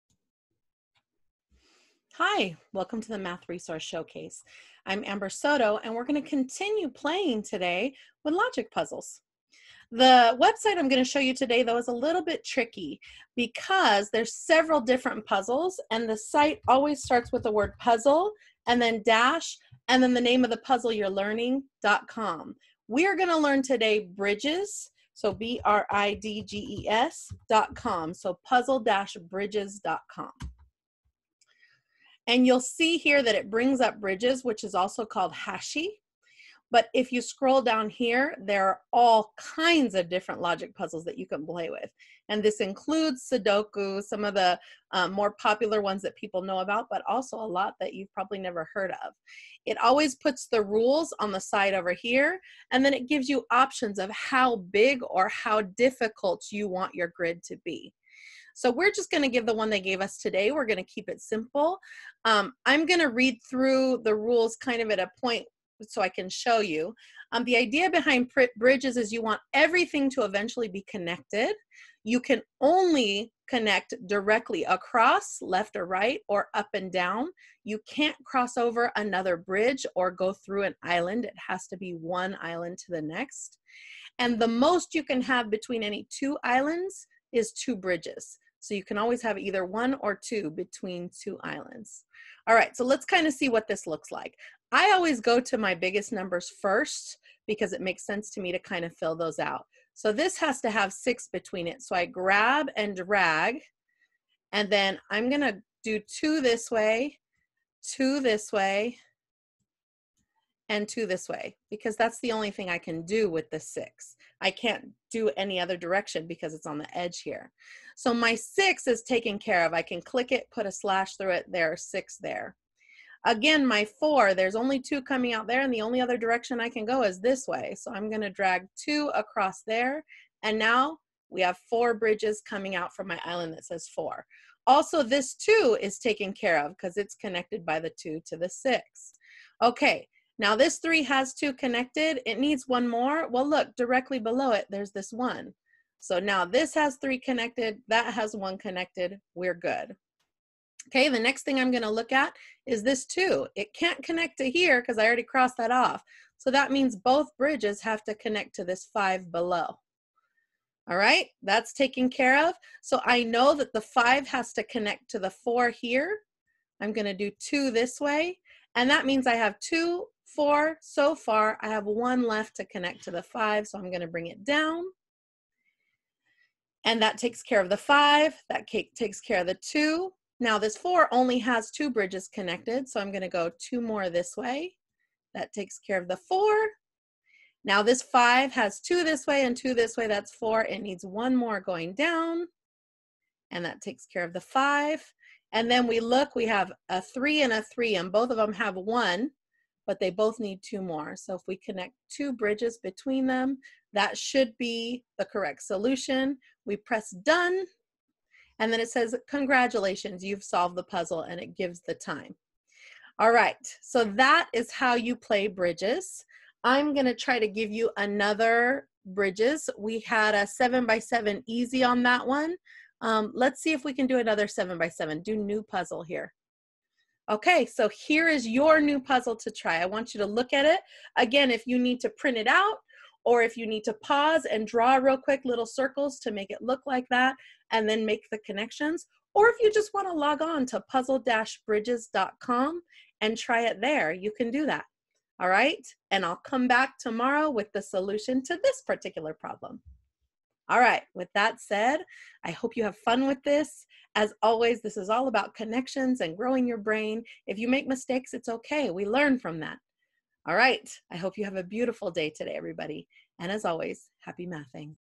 Math Resource Showcase - Logic Puzzles - puzzle-bridges.com
This is the next Math Resource Showcase Video focused on Logic Puzzles.  Today we visit a page: puzzle-bridges.com, but it is important to realize that you can put any puzzle name after the dash (for example puzzle-sudoku.com, or puzzle-skyscrapers.com, etc.).  Can you solve the Hashi (or Bridges) puzzle?  Have fun with this resource...and watch for the solution video to come out next!  Happy Mathing!  :)